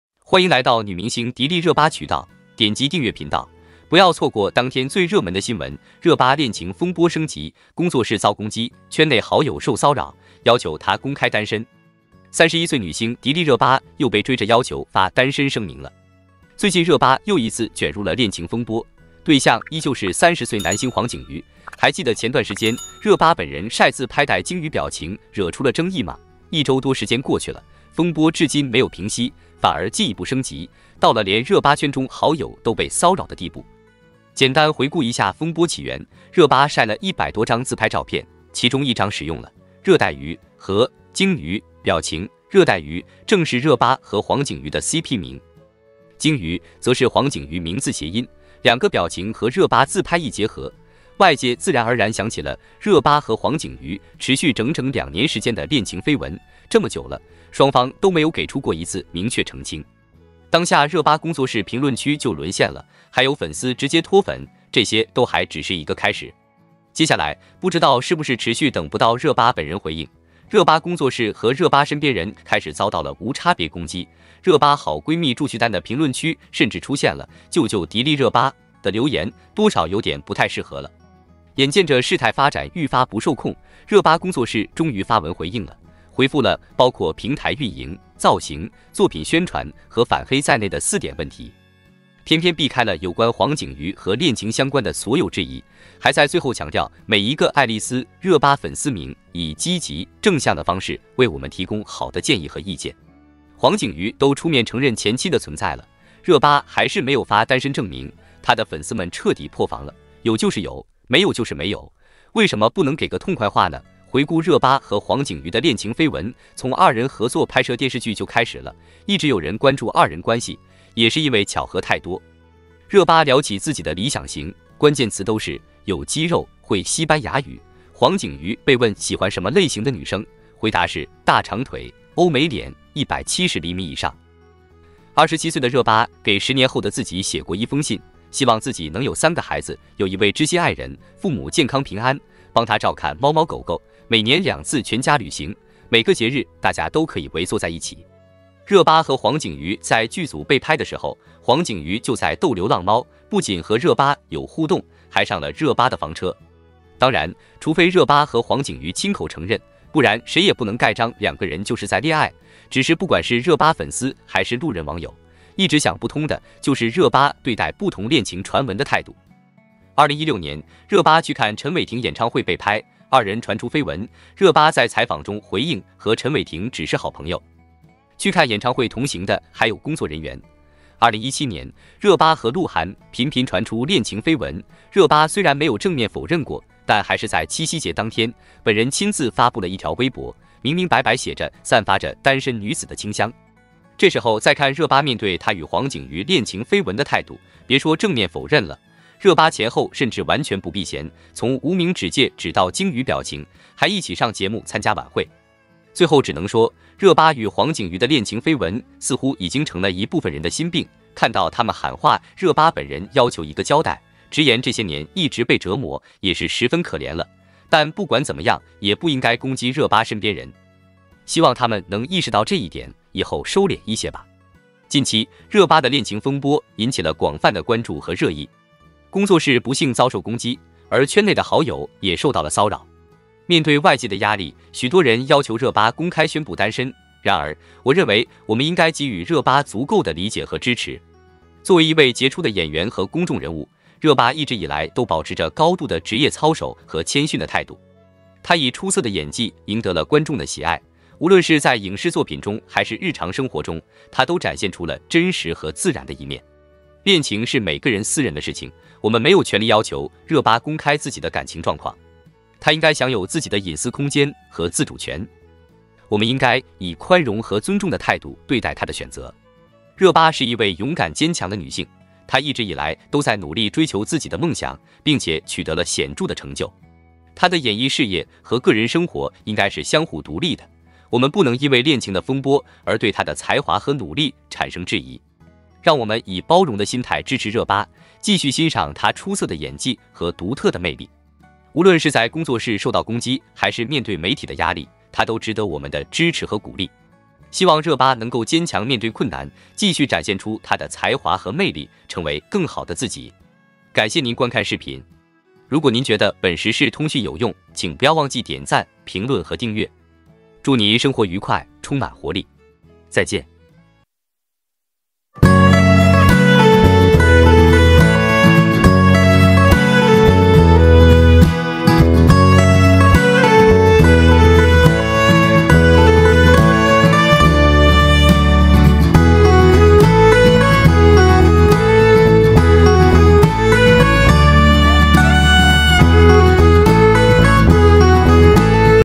【女明星6月26日】 迪丽热巴 恋情风波升级！工作室遭攻击，圈内好友受骚扰，要求她公开单身。【女明星迪丽热巴】
女明星迪丽热巴 -